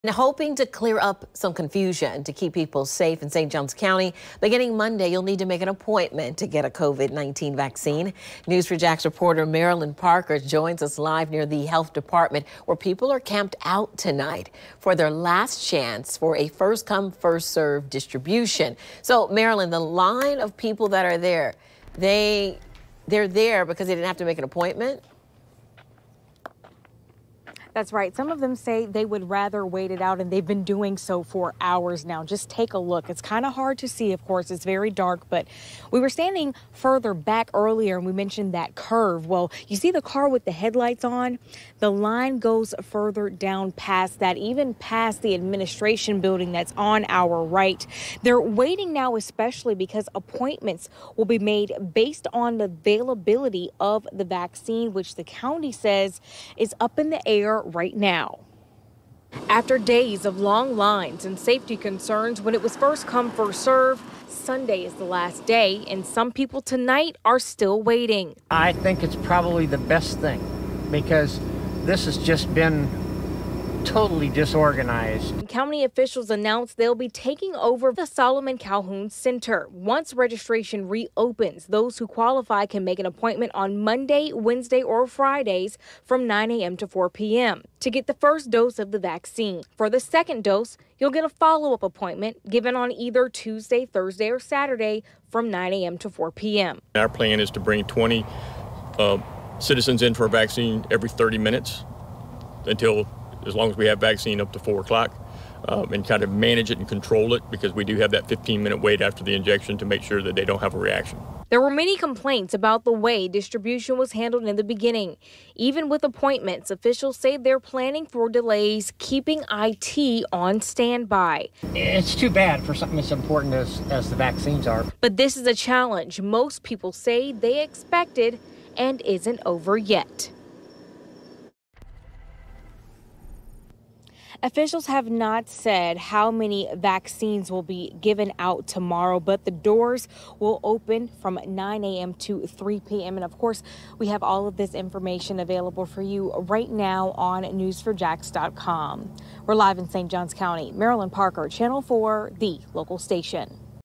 County taking over vaccinations from St. Johns health dept
Starting Monday you'll need an appointment to get the COVID-19 vaccine in St. Johns County.